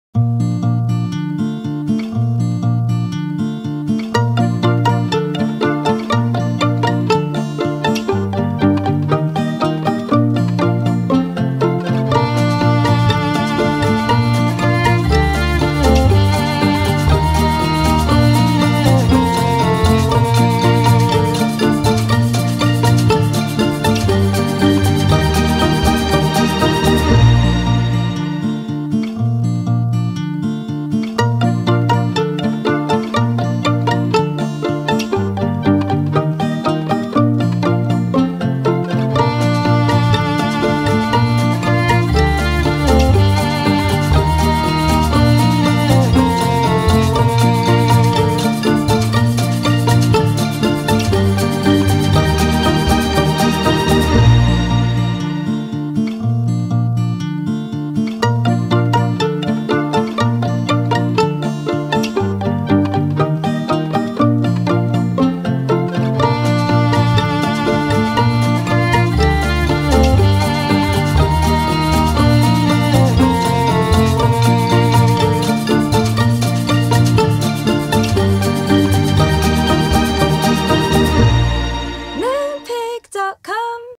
Moonpig PlantASmile 2015
A caption competition, comedians, flowers, the Edinburgh Fringe Festival and a whole lot smiles!